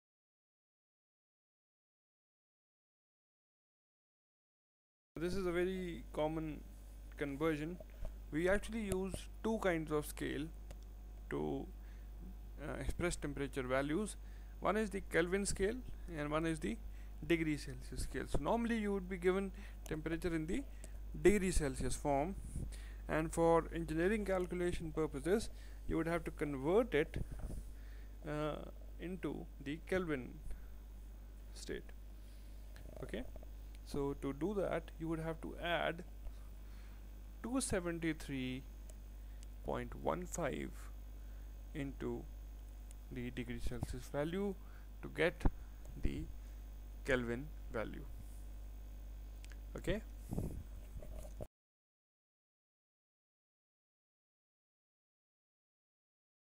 Kelvin Scale in temperature - Thermodynamics - GATE Mechanical Engineering lectures
Learn what is Kelvin scale in temperature. Learn the Kelvin scale. This video covers Kelvin scale and degree Celsius basics.